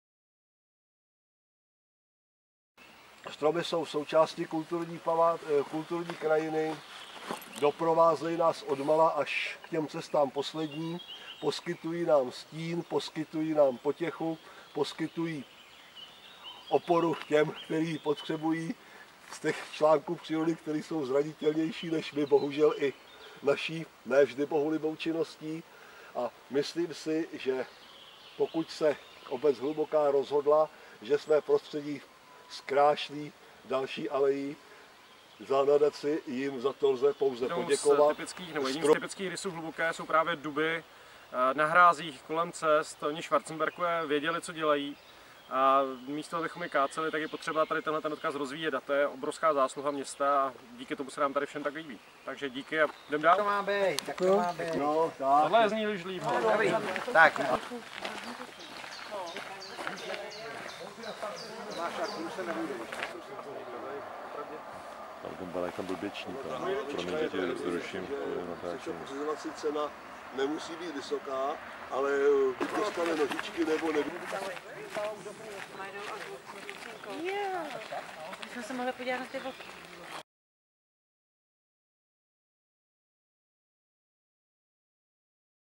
Výsadba dřevin Rudolfův kout
Město Hluboká nad Vltavou vysadilo na pěší stezce v Rudolfově koutě alej dubů za finanční podpory Nadace ČEZ v rámci programu Stromy.